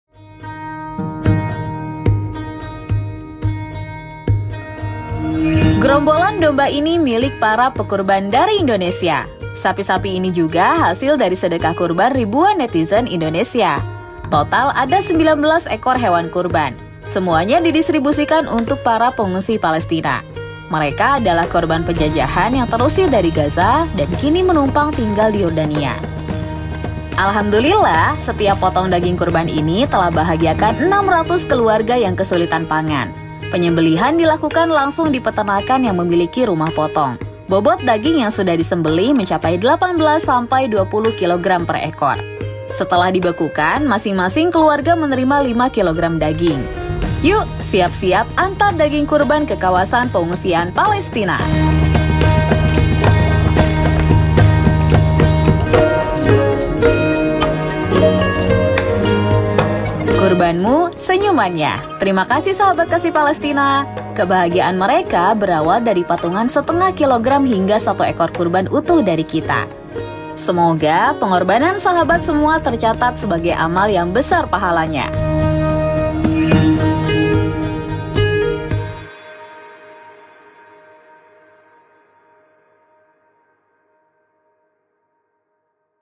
GEROMBOLAN SAPI & DOMBA ORANG INDONESIA UNTUK PENGUNGSI PALESTINA
Siapa sangka? Dari yang awalnya patungan ½ kg daging/orang, tiba-tiba ada 600 keluarga Palestina yang bisa merayakan Iduladha dengan bahagia.

Terima kasih Sahabat Kasih Palestina dan seluruh netizen Indonesia! Semangat gotong royong kita yang luar biasa, telah berhasil mewujudkan 19 ekor hewan qurban untuk para pengungsi Palestina.

Qurban disembelih dan dagingnya diantarkan ke kamp-kamp pengungsian Palestina yang ada di Yordania.

Mereka adalah pribumi Palestina yang terusir dari negerinya, dan kini hidup di negeri orang dalam kondisi sulit mendapatkan pekerjaan dan memenuhi pangan.

Semoga kebahagiaan Iduladha mereka tahun ini, menjadi sumber pahala yang besar untuk kita semua.

Qurbanku, Senyumannya🙂
Sampai jumpa lagi di Iduladha tahun depan ya!